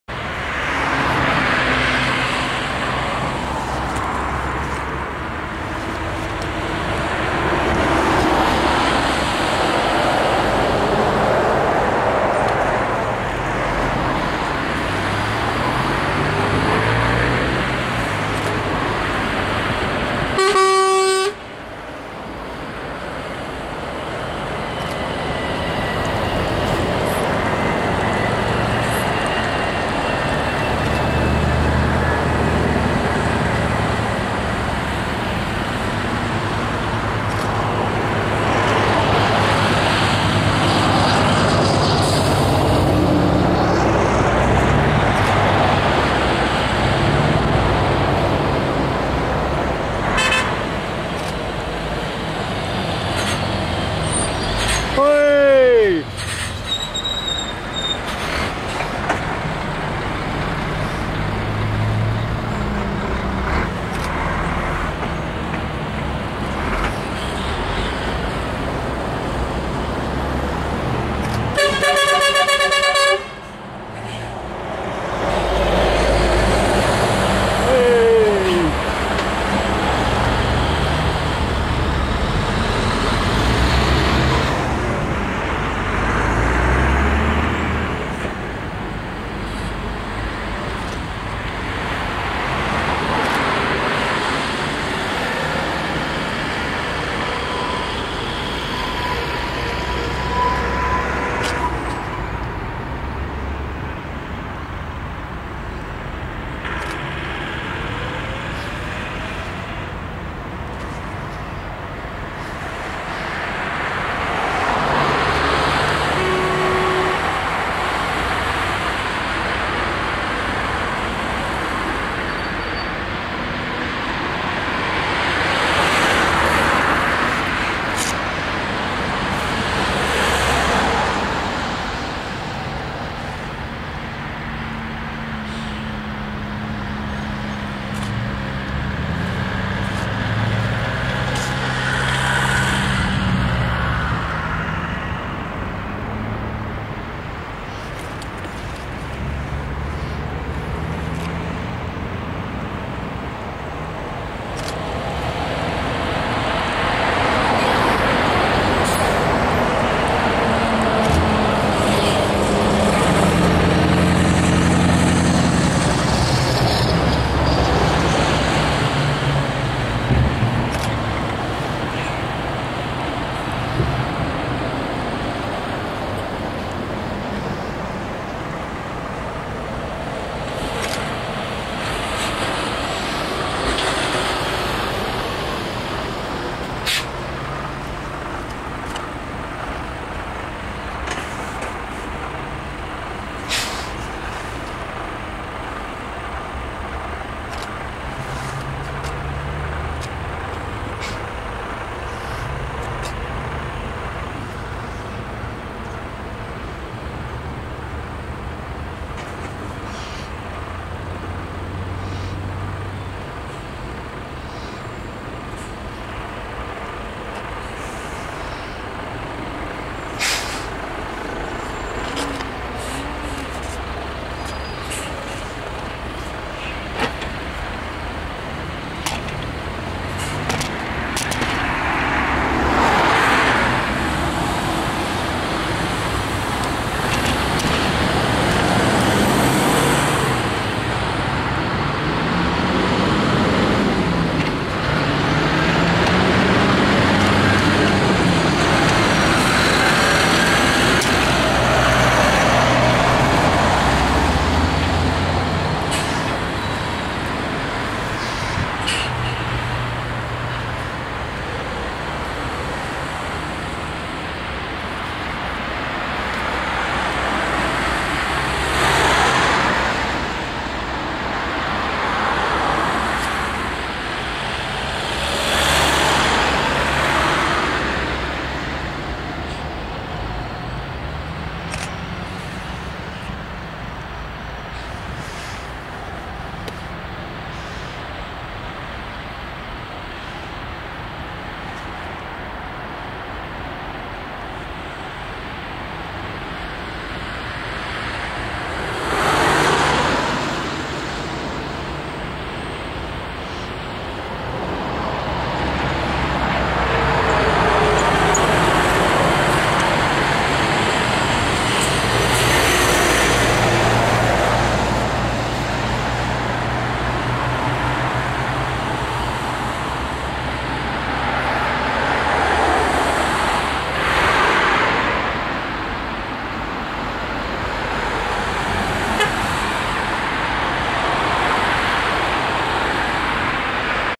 SKR convoy op weg naar Roemenië
Hulptransport SKR 2014 vertrokken richting Roemenië